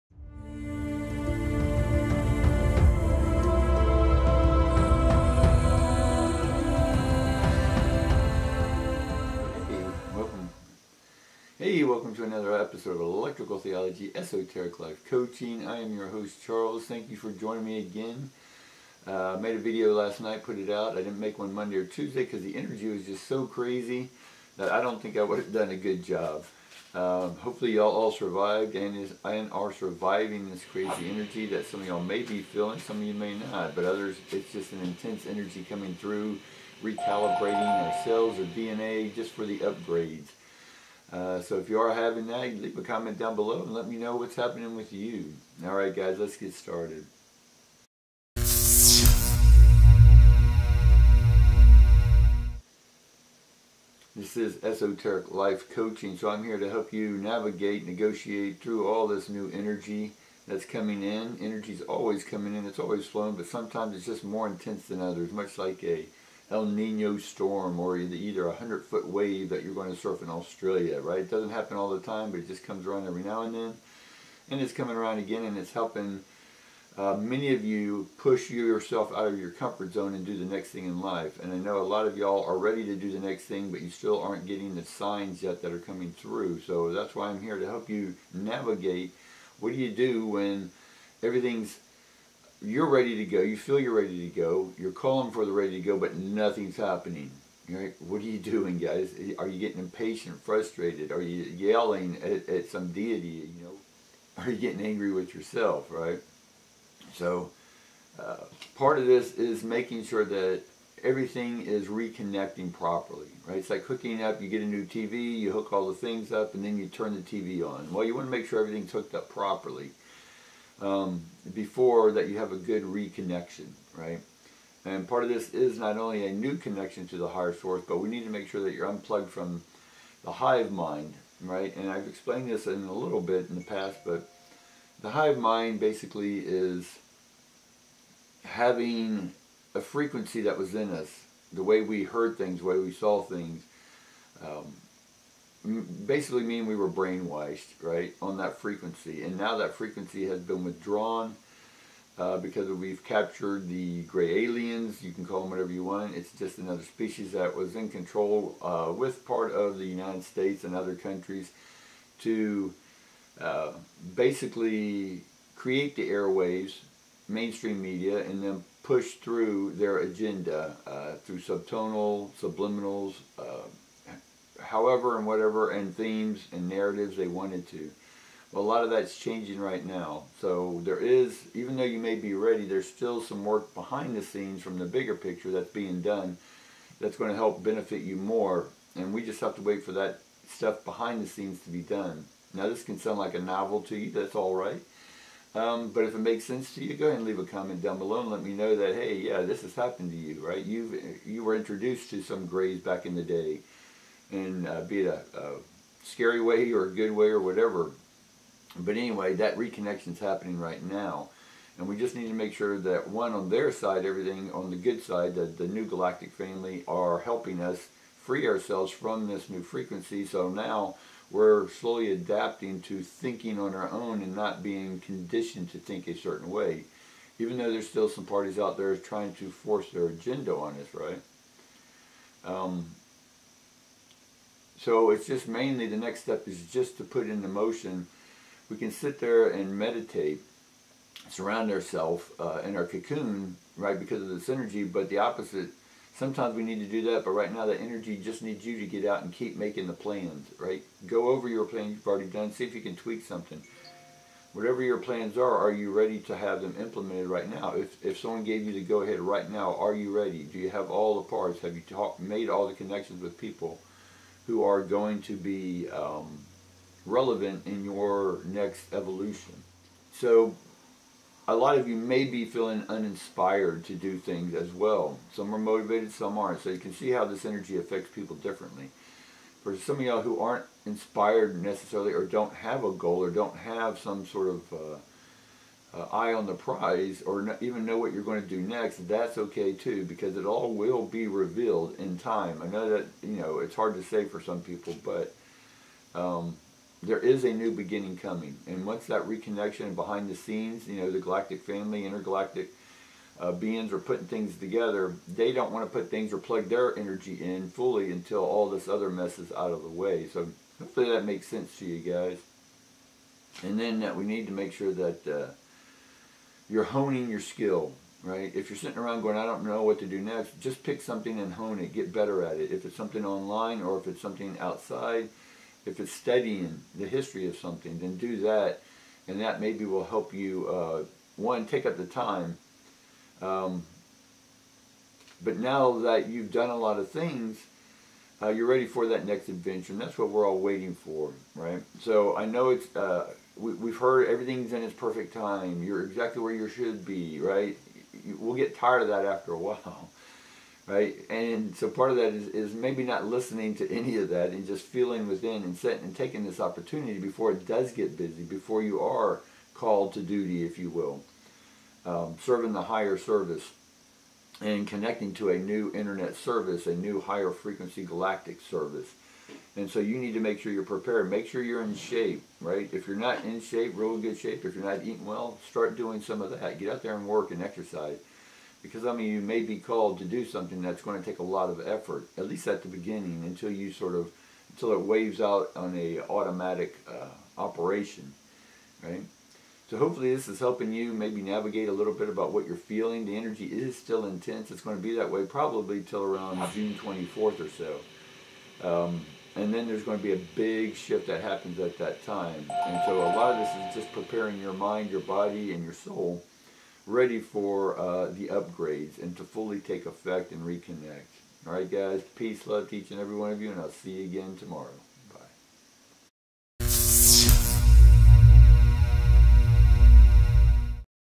TAROT ESOTERIC READINGS: THE HIVE MIND IS LEAVING
ELECTRICAL THEOLOGY ESOTERIC LIFE COACHING.

WE WERE CONTROLLED BY THE HIVE ALIEN MIND, BUT NOW WE HAVE CAPTURED THEM. FREEDOM IS COMING

ELECTRICAL THEOLOGY ESOTERIC SUPERNATURAL LIFE COACHING THURSDAY WK 29  MAY 25, 2023 ESOTERIC TAROT READING for the EVERYONE

***ARE YOU A SPACEC ALIEN???  9=} FOR A FREE FUN TOP 10 LIST TO SEE IF YOU ARE *** GO TO electricaltheology.com

for personal readings